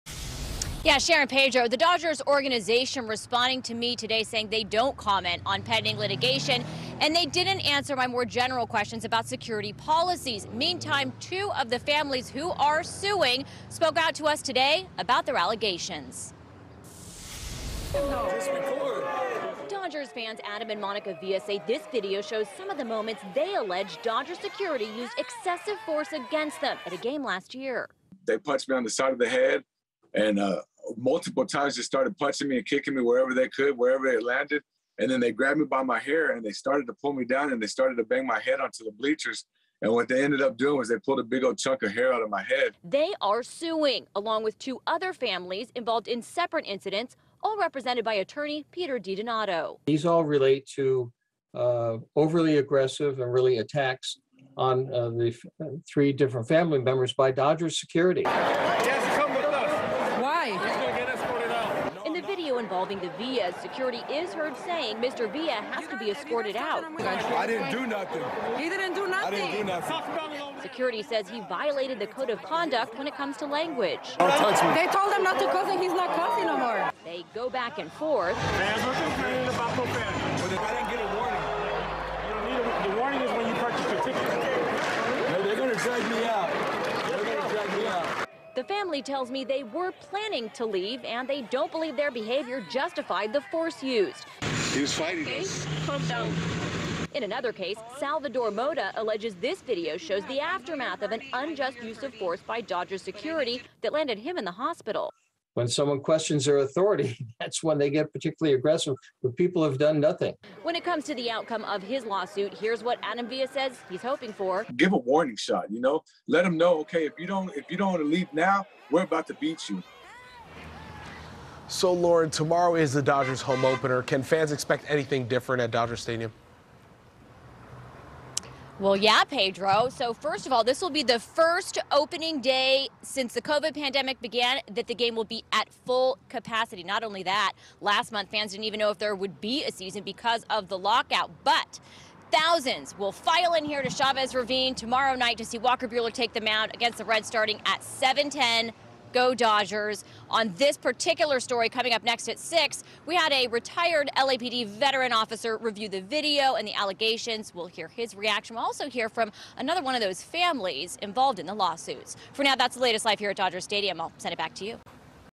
Dodgers fans allege brutal treatment by stadium security
Lauren Lyster reports for the KTLA 5 News at 5 on April 13, 2022.